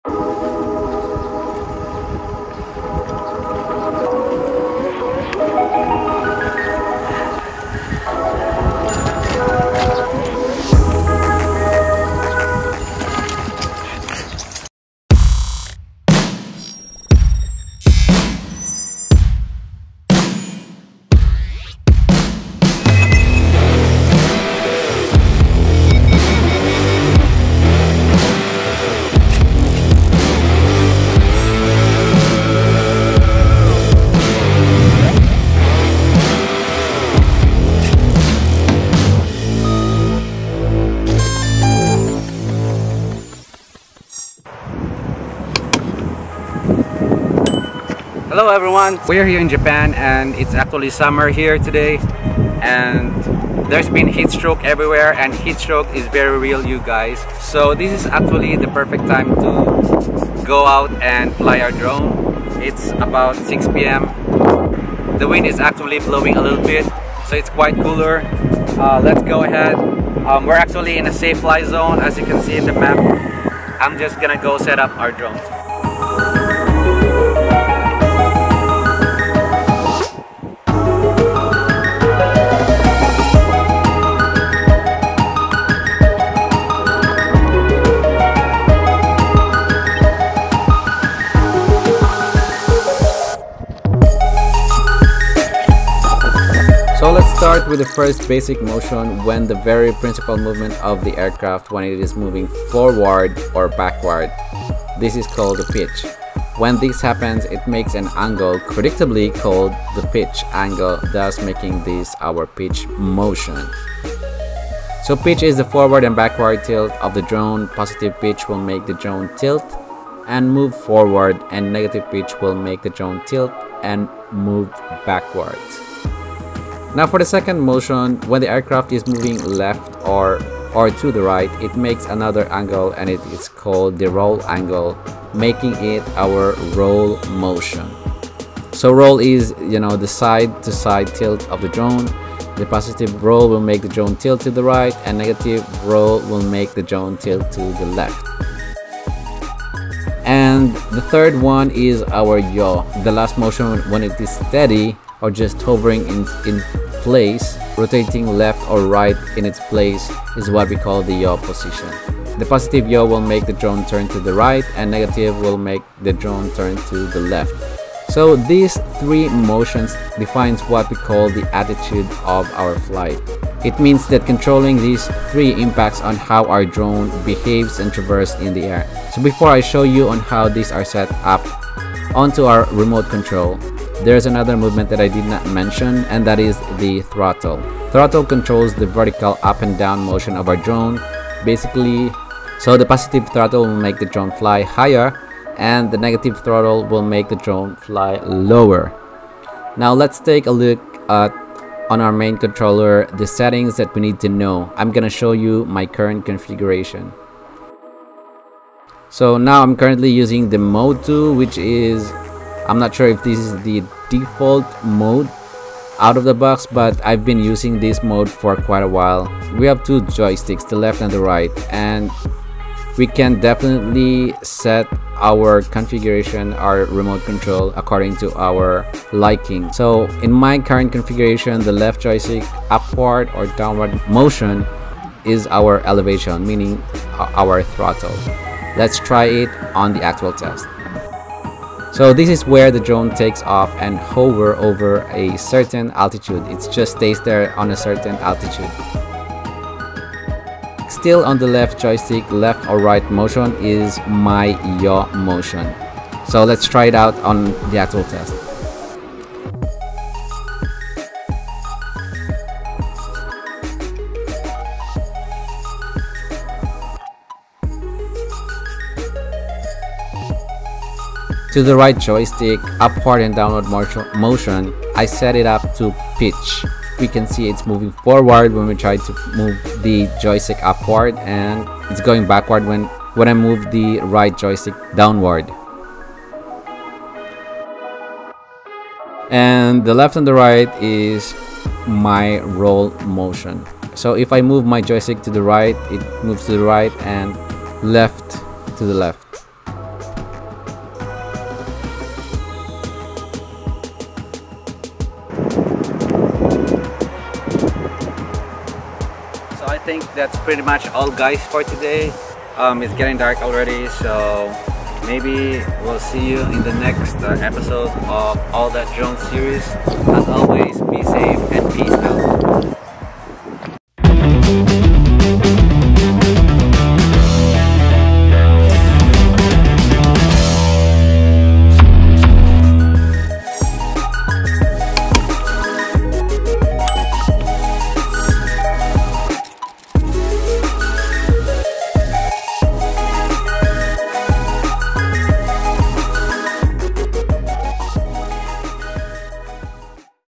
Understanding UAV Pitch Roll Yaw Basic Movements
All That Drone | Episode 03

Timeline
0:00 Intro
1:38 Basic Motions

Episode 03 is when we actually fly our drone. This is all about the basic movements straight out of the box. I had these 3 motions checked first when I first open my drone back in 2015, And without going deep into any tutorials then, I could definitely fly it by carefully testing out those joysticks.
So what are these motions - pitch, roll and yaw.

Warning!!!

📷Sony a7iii
📷 Camera: Canon EOS M6